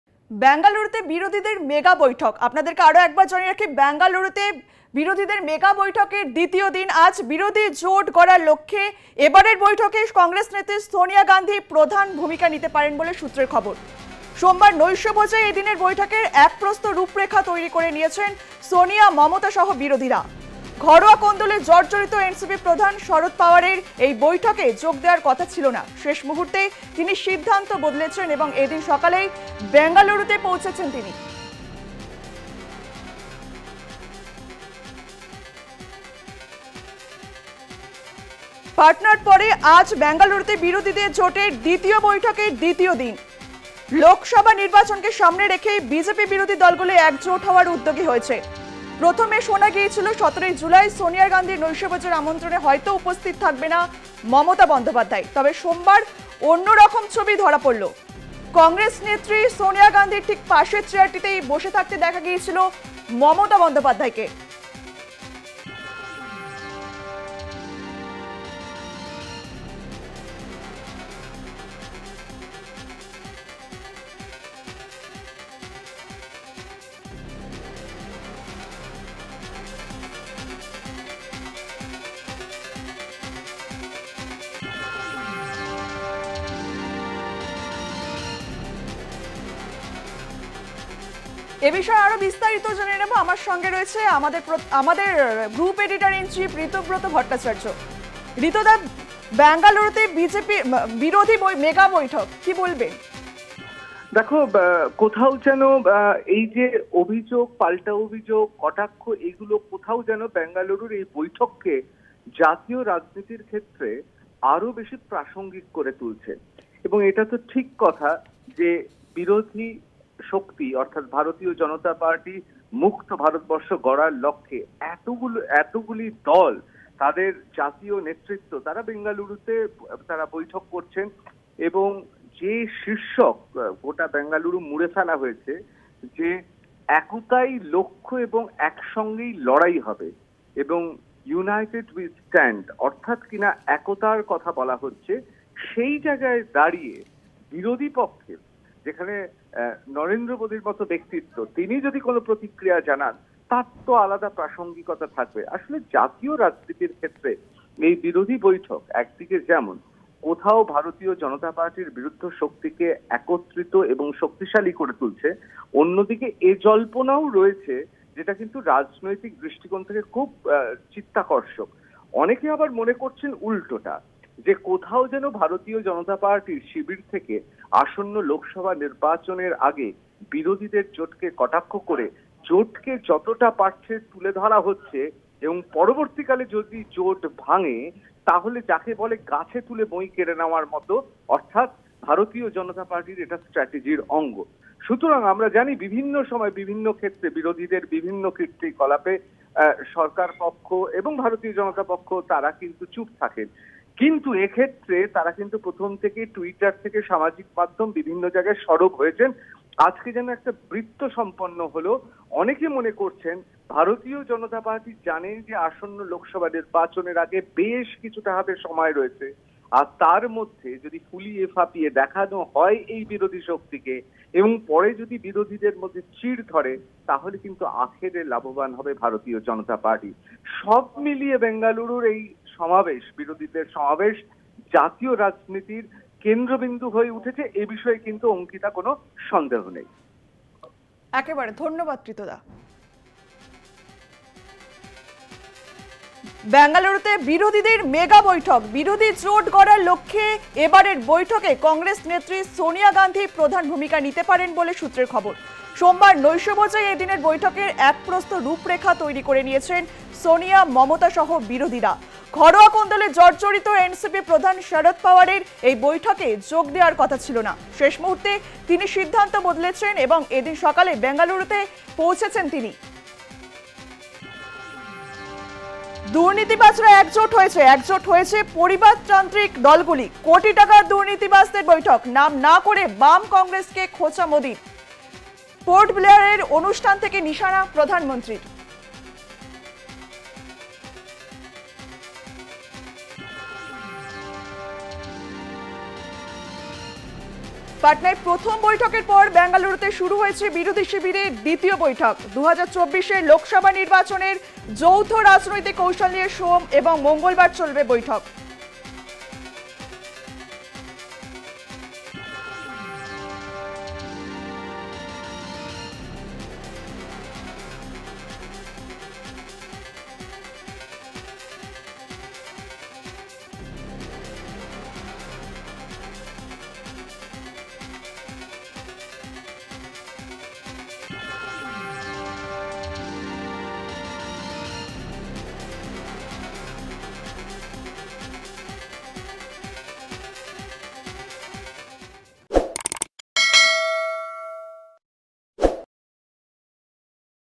Opposition Alliance Meeting: বিজেপি বিরোধী ২৬ টি দলের মহাজোটের বৈঠক বেঙ্গালুরুতে ।R Plus News
Opposition Alliance Meeting: বিজেপি বিরোধী ২৬ টি দলের মহাজোটের বৈঠক বেঙ্গালুরুতে ।